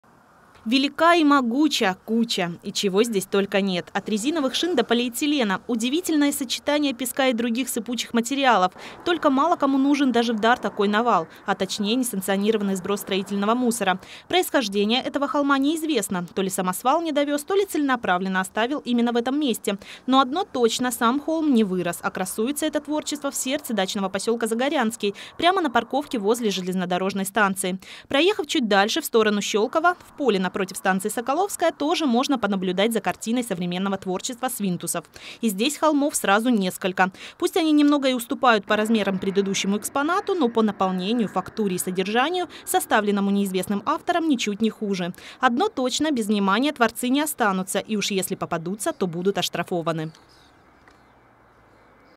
Хочу показать: мусор на Загорянке
Парковка возле платформы Загорянская закреплена за Центральной пригородной пассажирской компанией. Администрация городского округа уже направила заявление в Госадмтехнадзор, в ближайшее время будет проведена проверка на месте несанкционированных сбросов.